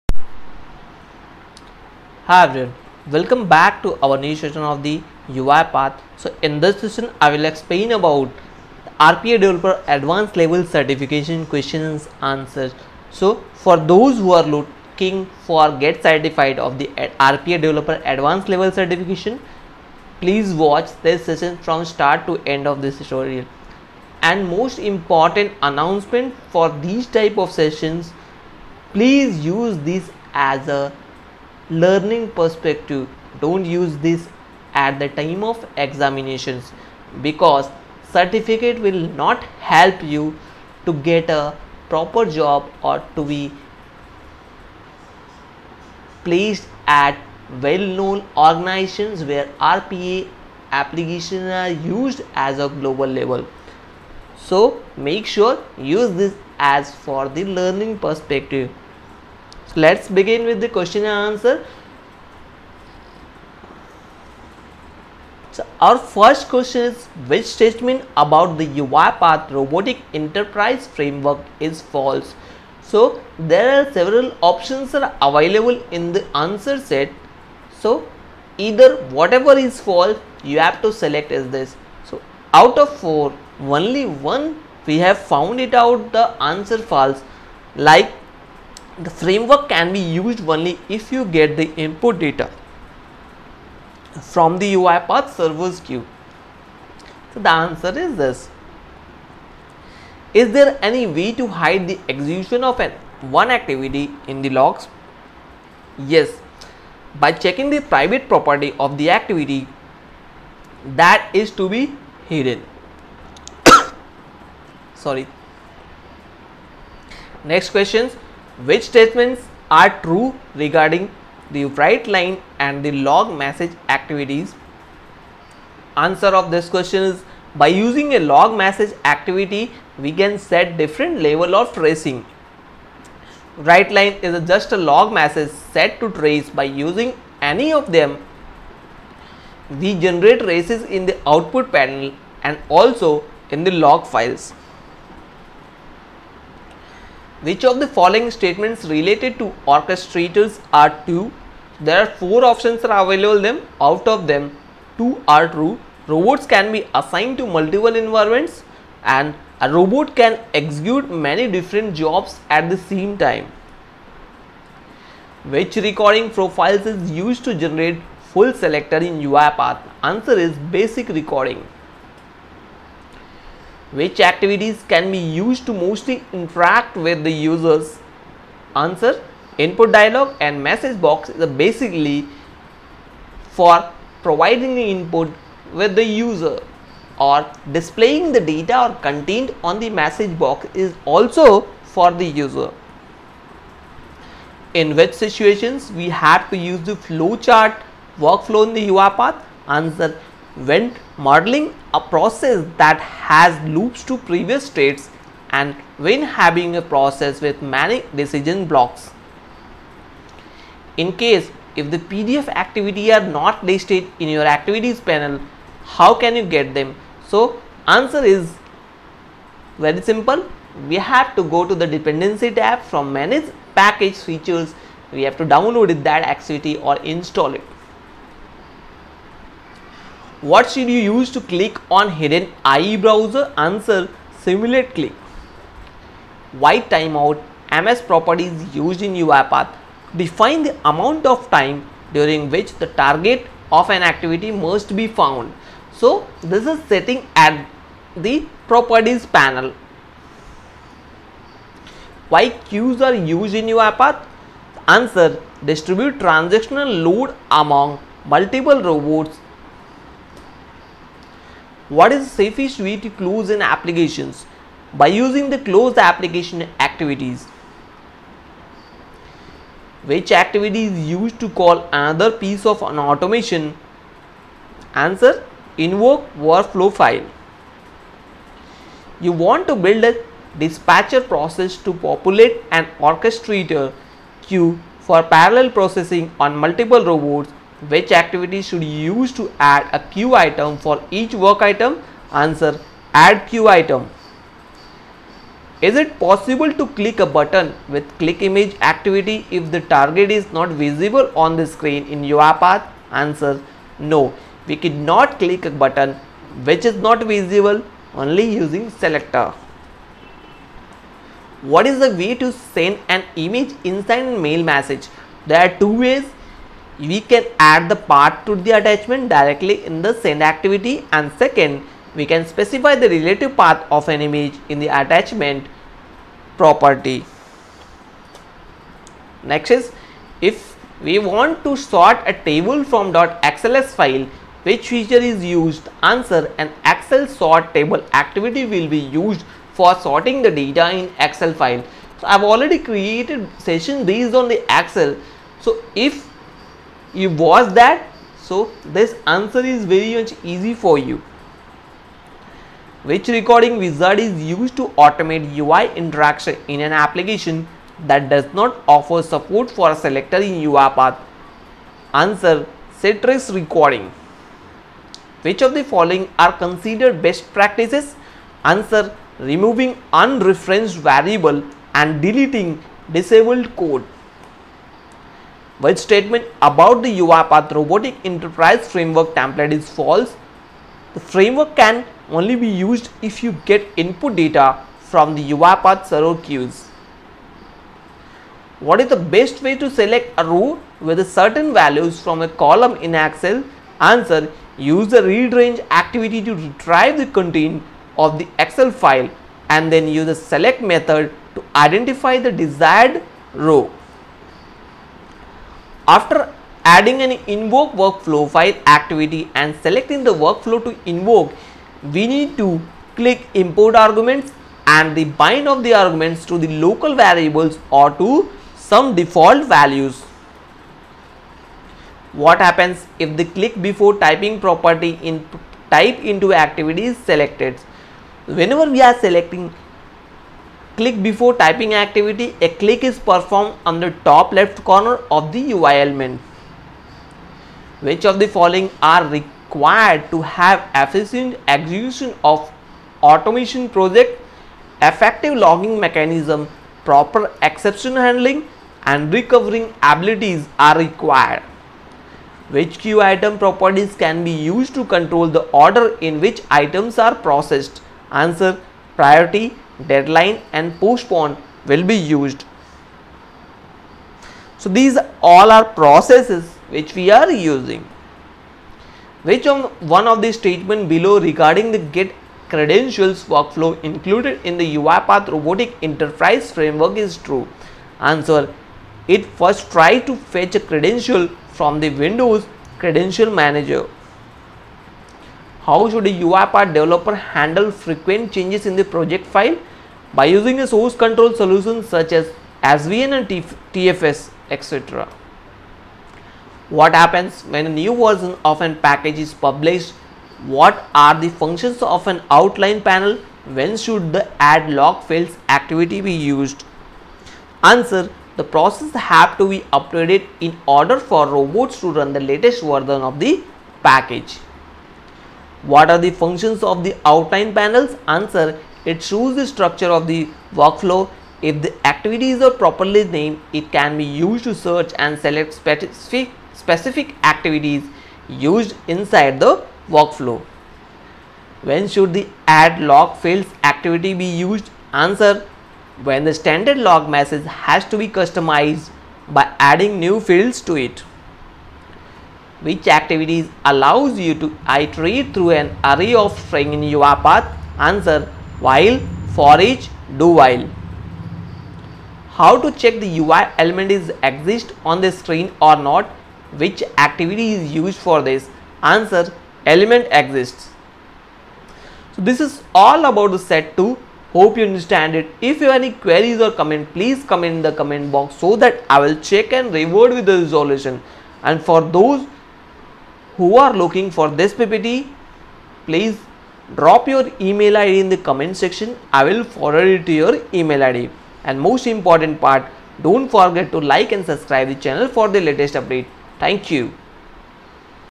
Uipath RPA Developer Advance Level Certification Question and Answer Set - 2
Based on User Request I created session on Uipath RPA Developer Advance Level Certification covered almost all the possible question which will asked at the time of certification.

Use this question and answer for the preparation of Certification and not at the time of examination:

1. How many types of actions can be performed in the Variables panel in UiPath?
2. What is the possible technique to get the content of a PDF document is available in UiPath?
3. How can a robot start an application in Citrix in UiPath Studio?
4. During the running of workflow, how can you see the steps the workflow is executing?
5. Can you store a Selector in a variable?

Hope this will help you for the certification program... All the Best.